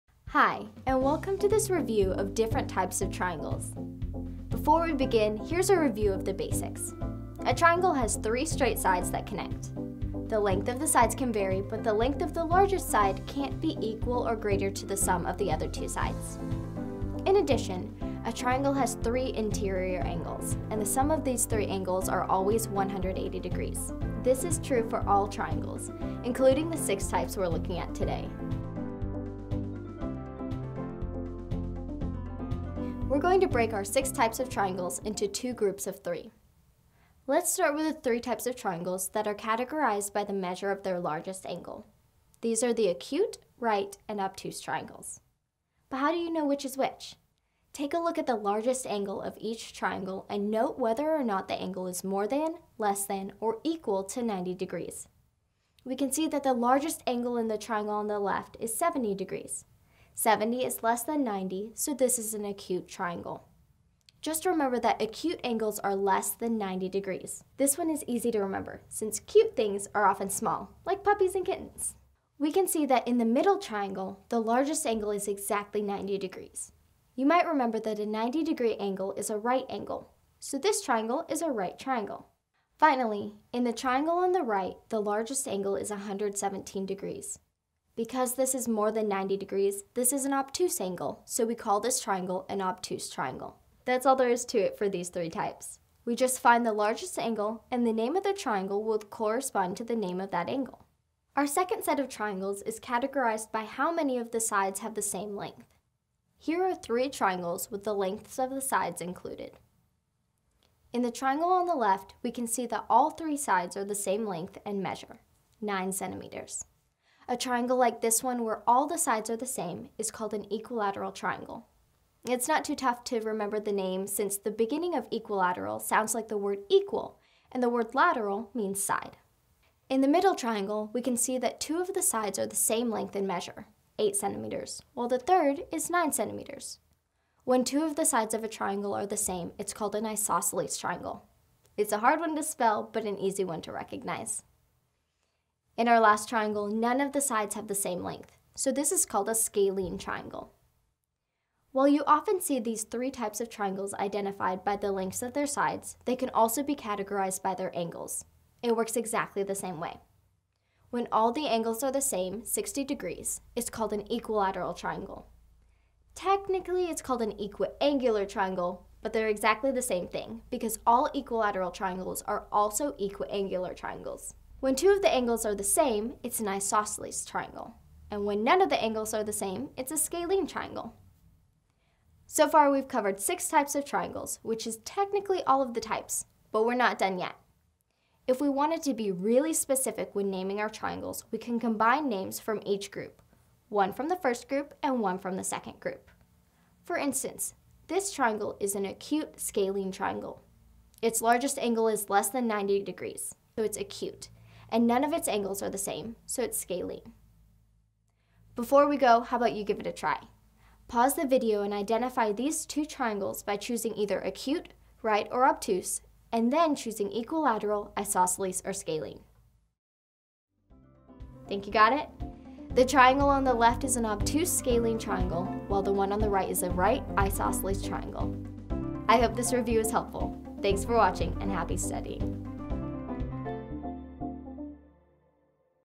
Types of Triangles
Hi, and welcome to this review of different types of triangles! Before we begin, here’s a review of the basics. A triangle has three straight sides that connect. The length of the sides can vary but the length of the largest side can’t be equal or greater to the sum of the other two sides. In addition, a triangle has three interior angles, and the sum of those three angles is always 180 degrees.